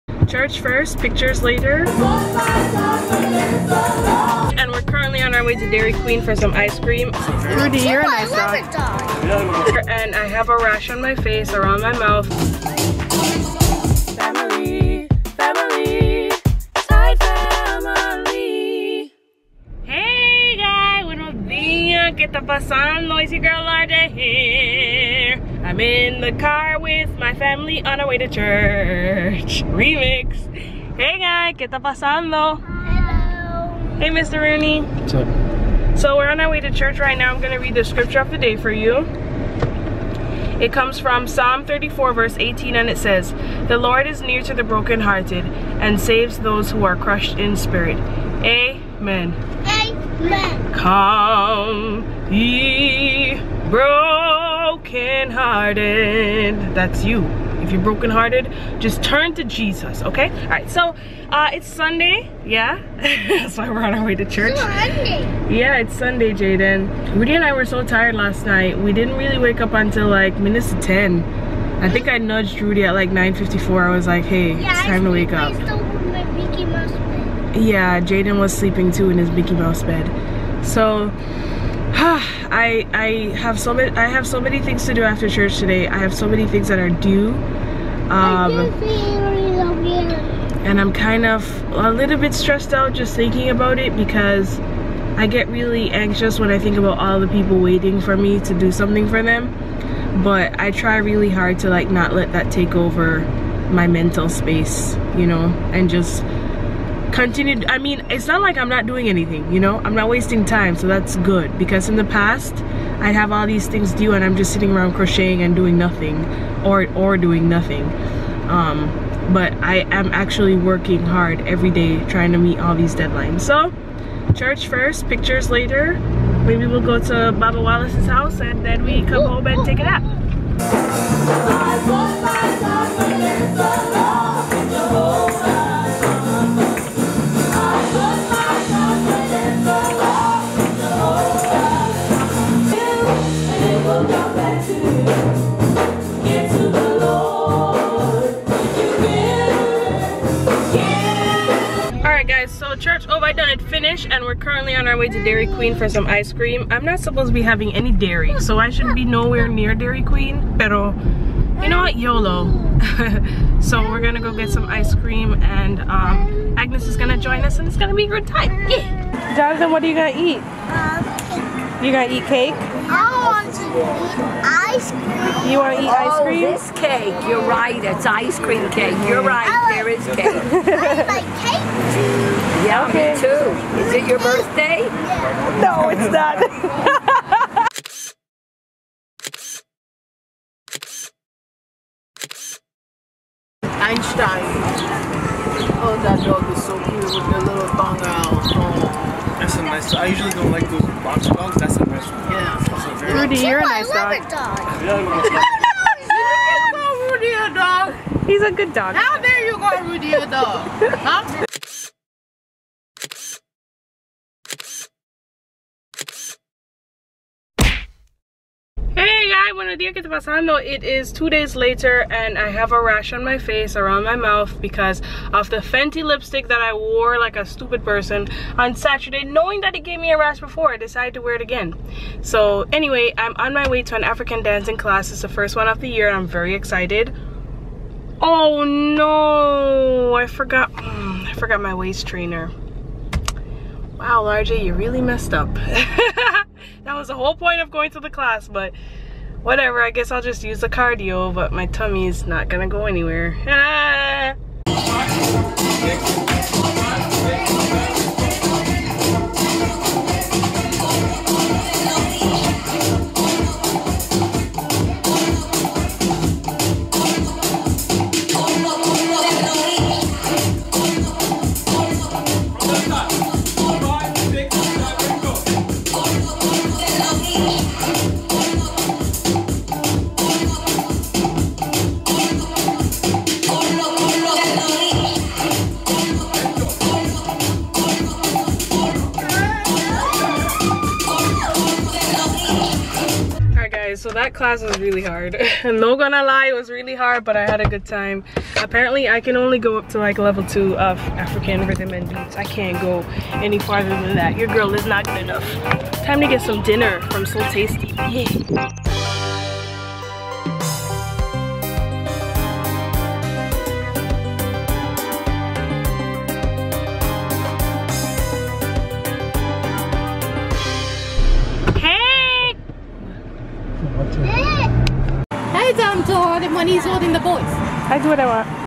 [S6E107] : Ice Cream Date & African Dance Class! [5.19.19 & 5.21.19]
In this vlog, we went to church then went to Dairy Queen with Agnes & Kayly, ate ice cream, then took some portraits. The next day, I had a good time at Afro R&B class.